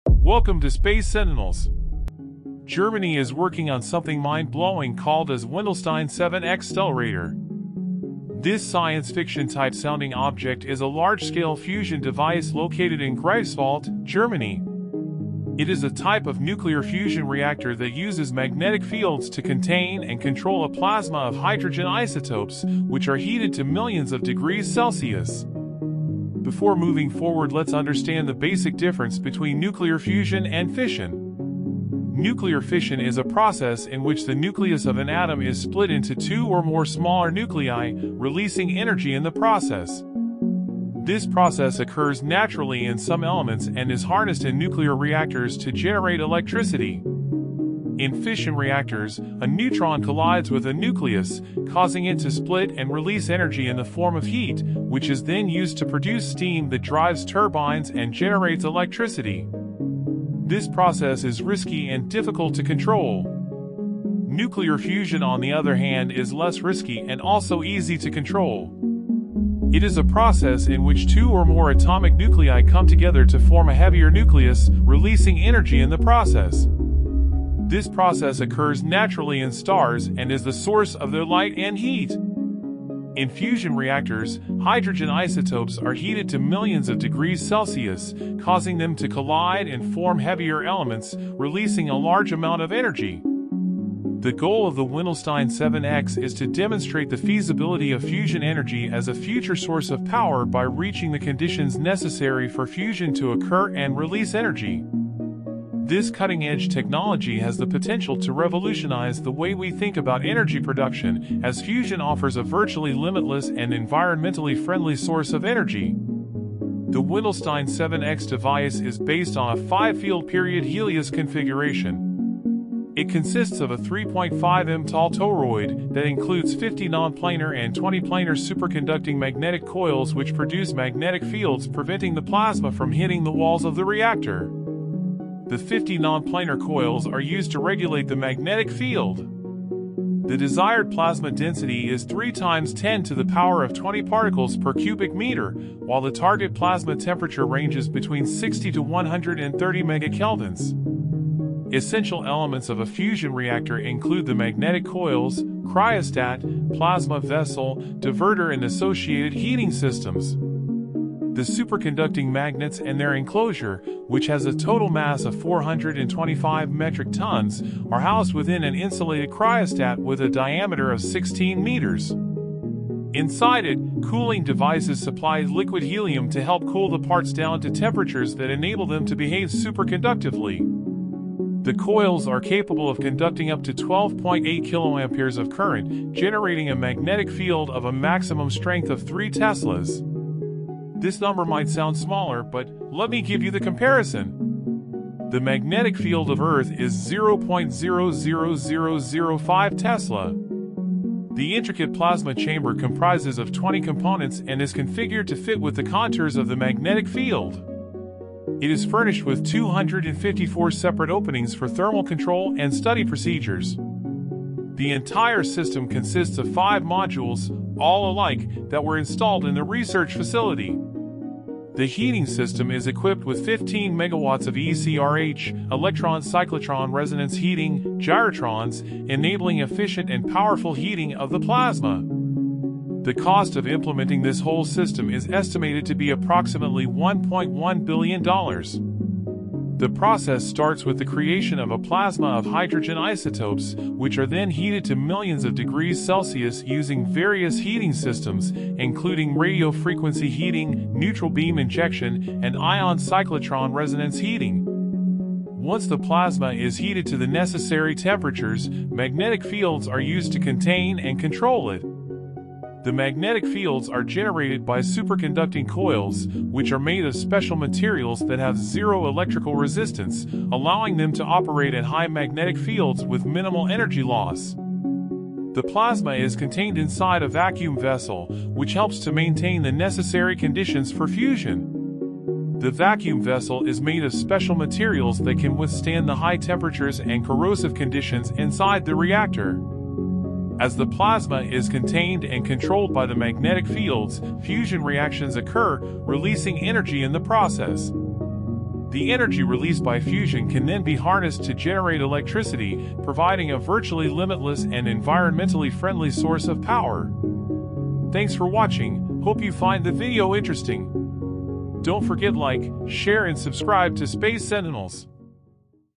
Artificial Sun. This is Insane. Wendelstein 7X Stellarator.
Germany never fails to startle the world. Wendelstein 7-X (W7-X) is a large fusion research device located in Greifswald, Germany. It is a type of fusion reactor called a Stellarator and it is designed to produce energy through fusion reactions between hydrogen isotopes. The device uses magnetic fields to confine hot plasma in a controlled manner, with the goal of achieving a stable fusion reaction that releases more energy than it consumes. W7-X has a unique, complex design that aims to simplify the challenges faced by other fusion reactors, making it one of the most advanced fusion research devices in the world.

Thankyou!